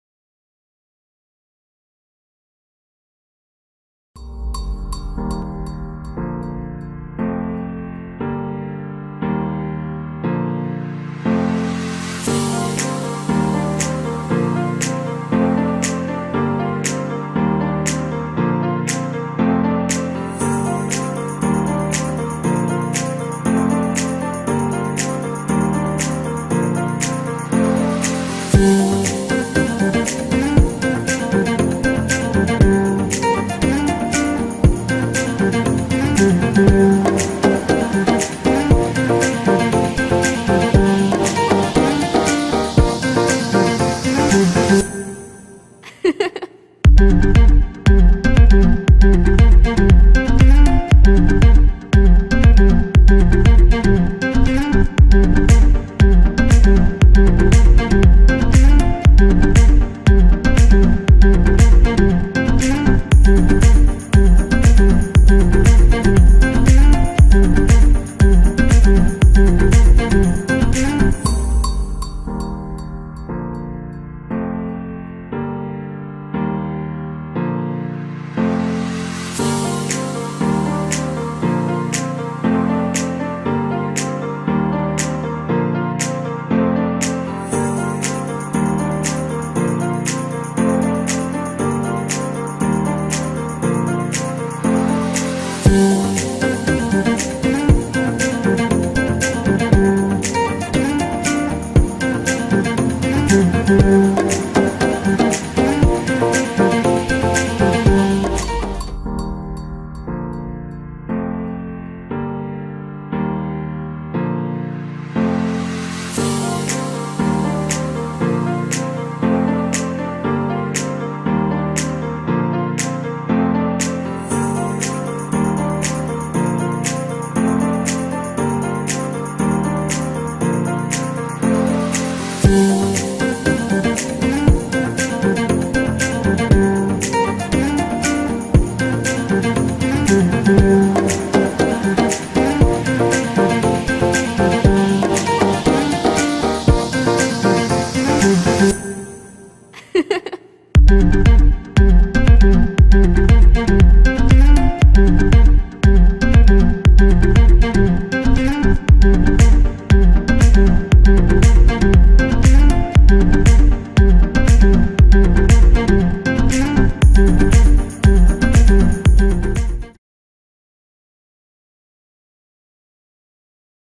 PARIS 4 ST PAUL
Tres beau dyplex ultra calme et idéalement situé au coeur du Marais
bien calme et lumineux bénéficiant de beaux volumes
Rare